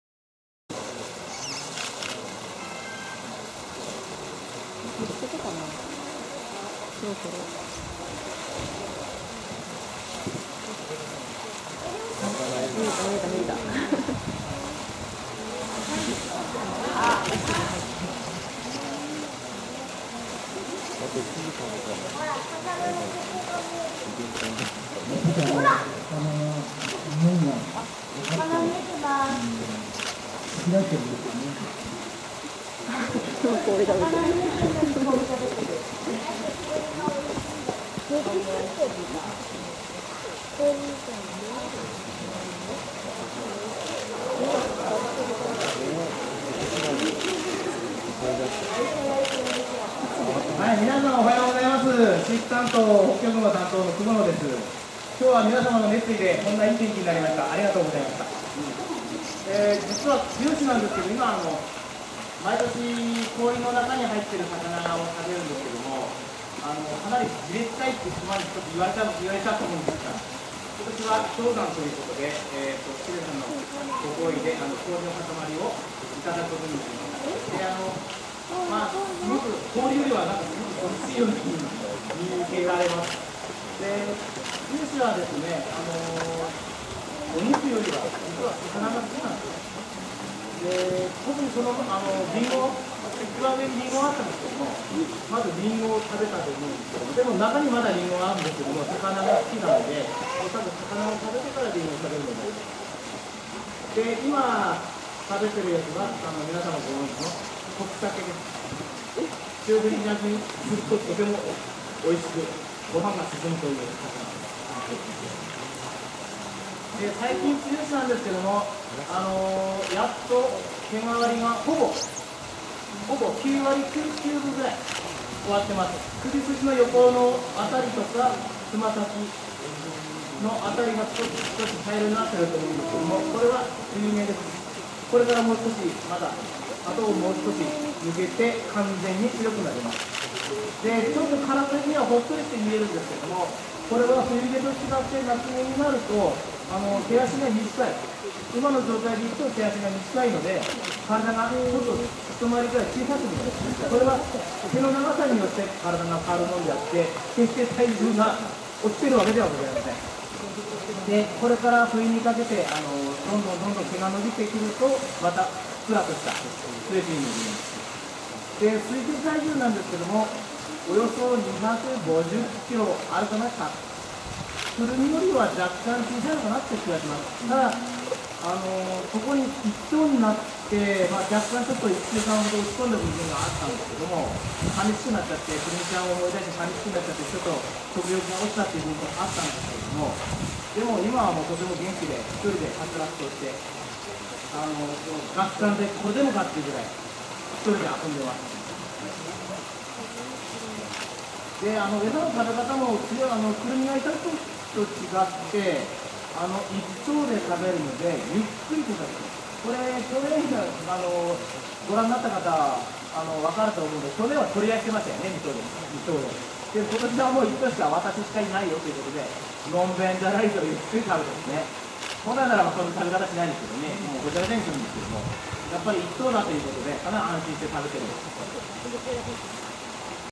2011年7月24日 釧路市動物園 時知らずを食べるツヨシ・・・１
釧路市動物園で開催された、発泡スチロールの日イベントの様子です。
放飼場に氷の山が作られ、その中に時知らずやリンゴが隠され、ツヨシが食べている様子です。